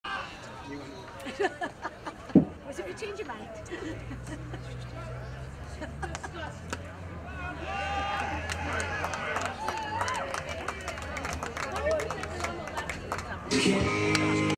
Muskegon Clippers vs Cincinnati Steam
3rd Inning “Muskegon” 1st batter,  hit by pitch